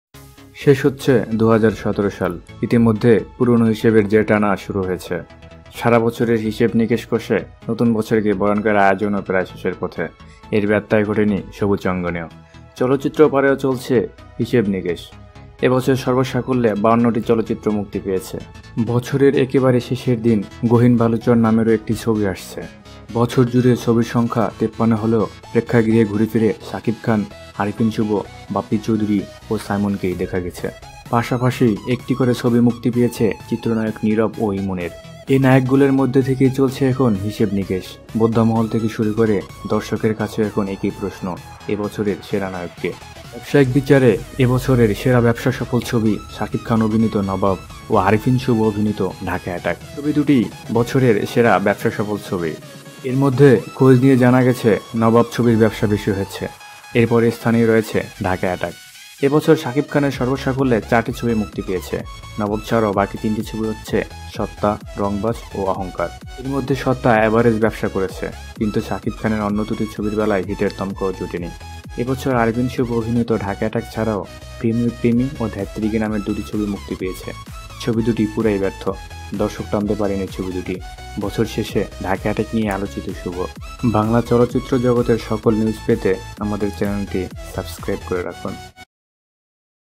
Best Bangladeshi movie of 2017/ Nabab/ Dhaka Attack/ Shakib Khan/ Shuvo
52 movies released  in this year in Bangla film industry. But most of all was flop.

Only Shakib Khan's Nabab and Arifin Shuvo's Dhaka Attakc was very successful movie in this year.